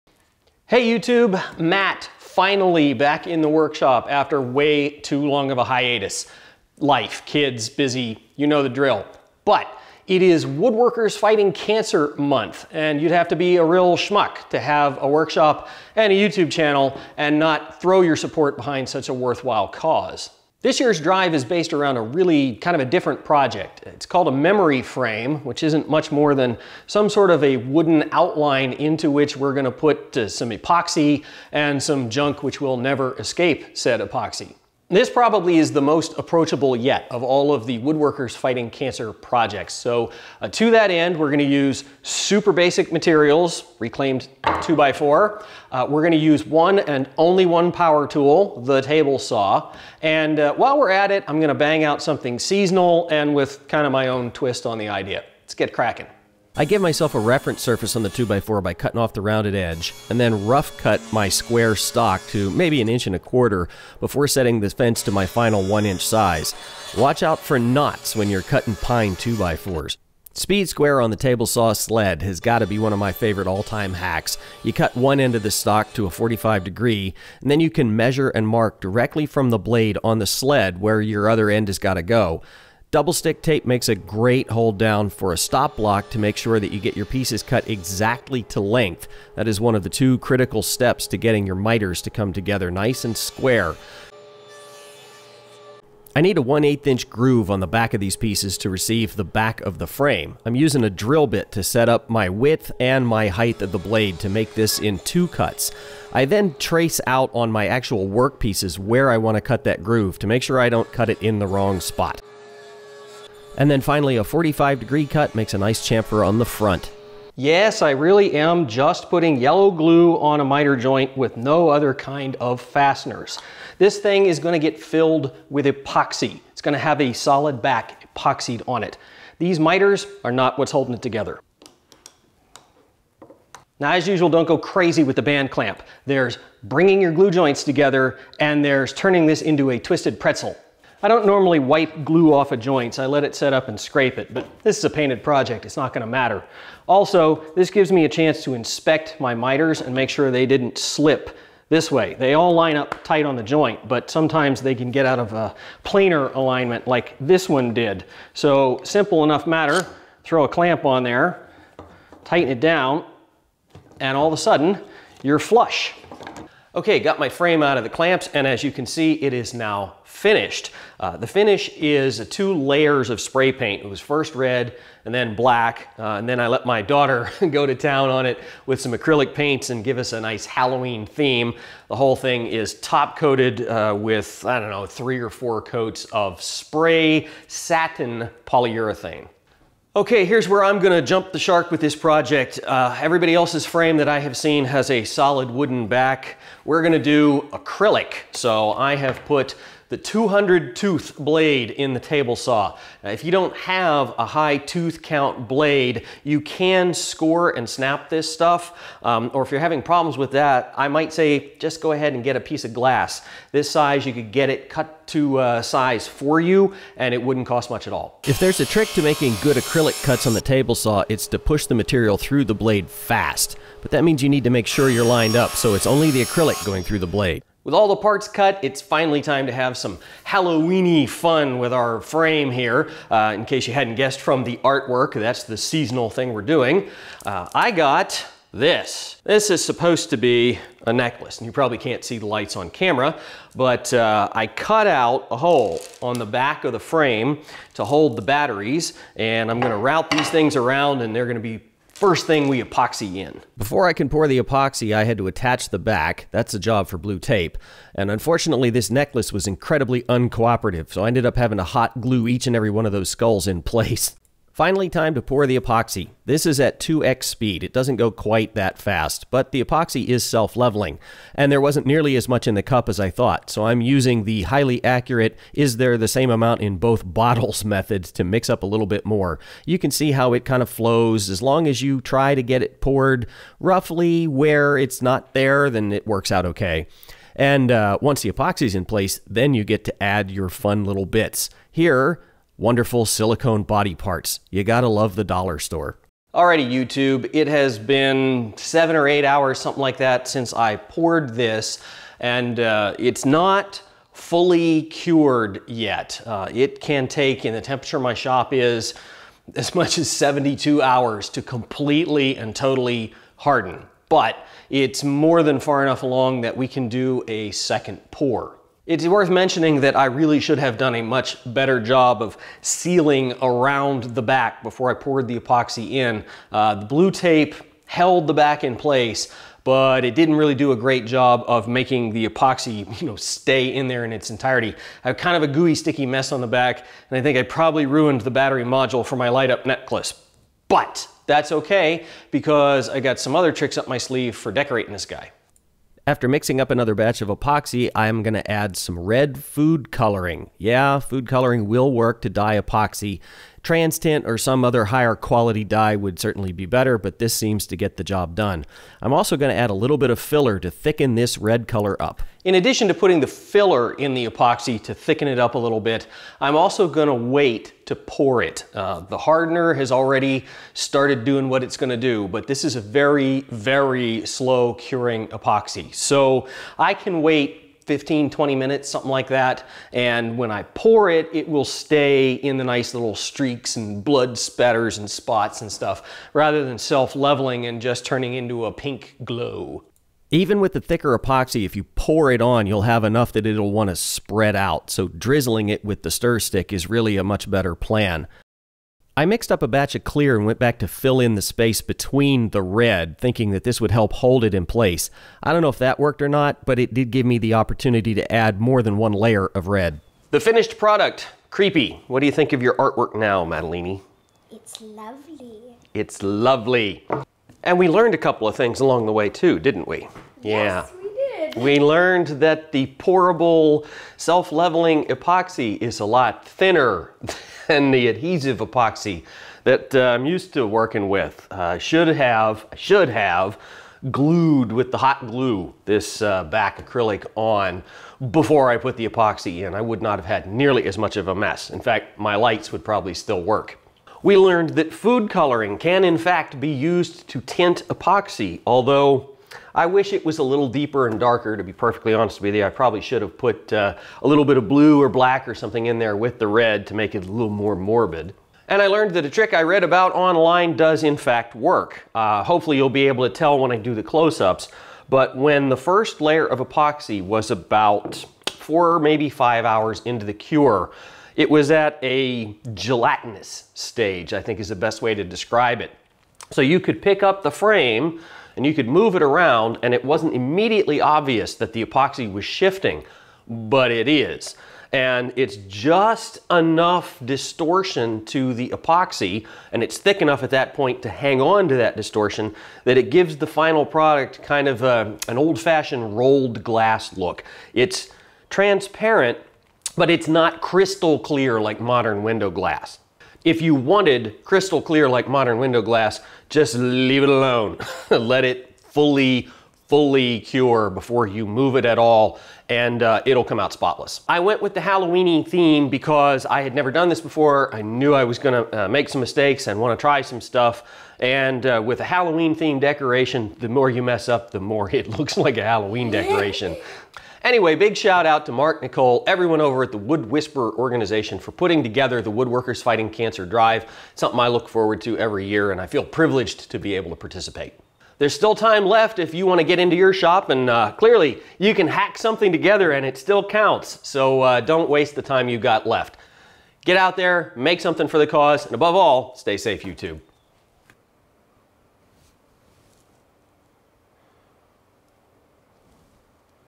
WWFC 2017 Halloween
It's woodworkers fighting cancer time again.  I love this event.  It's for a great cause and the projects are always a blast to make.

I decided to Halloween-up my memory frame since it's that time of year.  Also happens to be a great excuse for all of the mistakes I made.